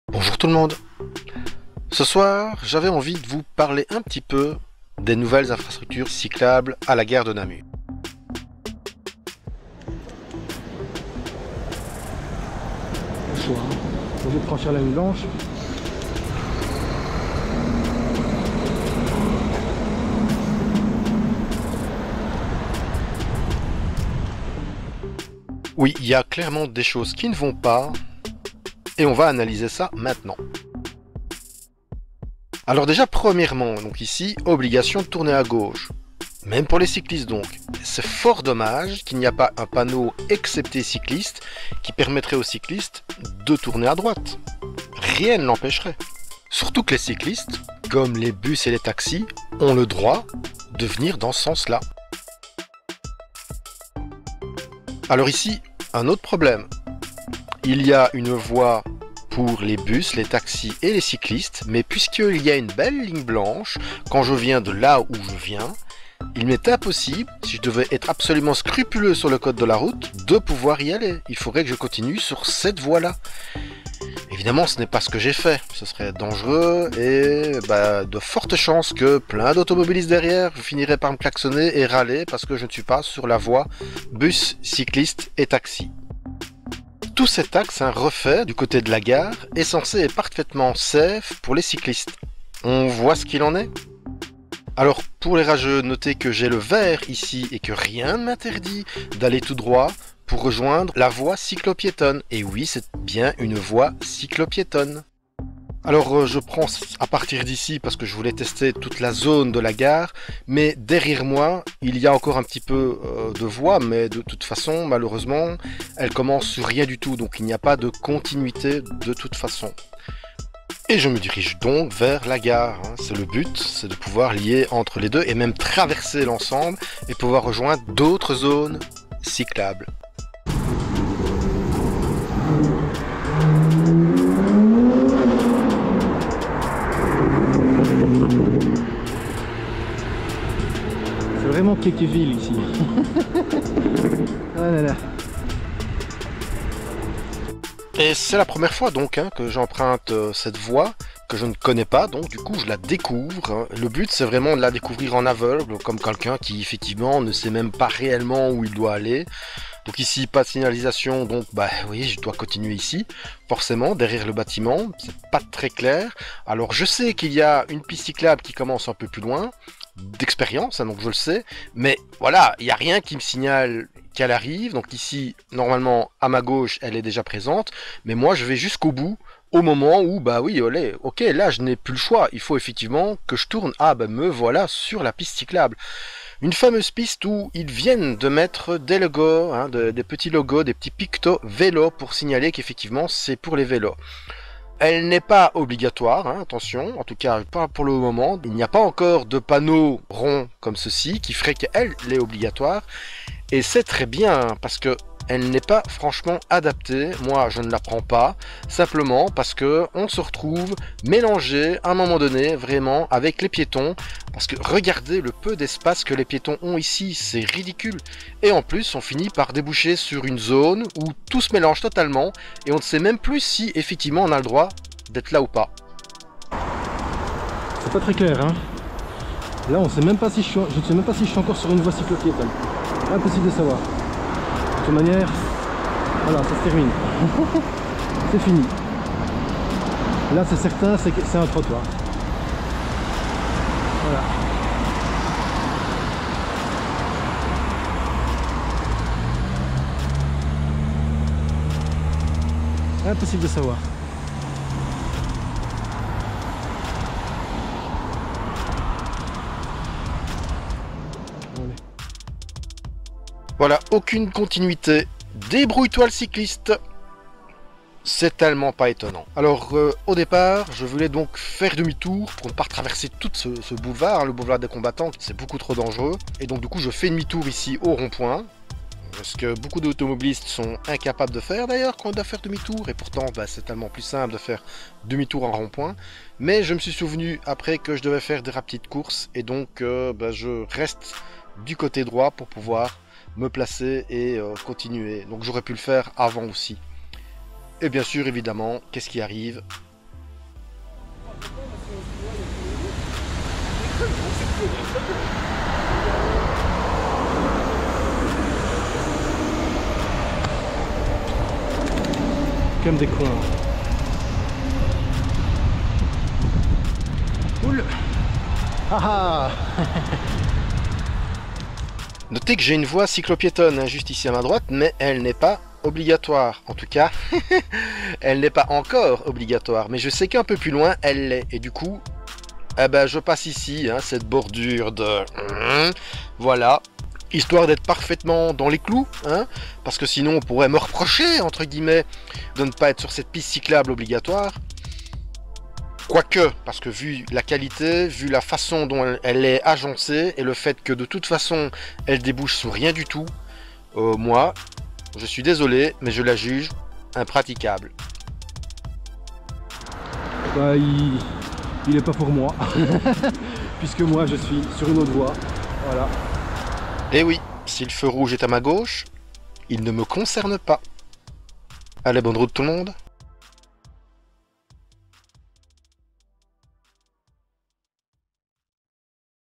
La nouvelle place de la gare de Namur : Peut mieux faire !
Filmé le 31 juillet 2024

Vélo : Azub Six et vélo de course
Caméra : GoPro 7, Osmo DJI action et Eken H9
logiciel de montage : AVS vidéo éditor

Musiques : Proposées par Youtube, merci. :)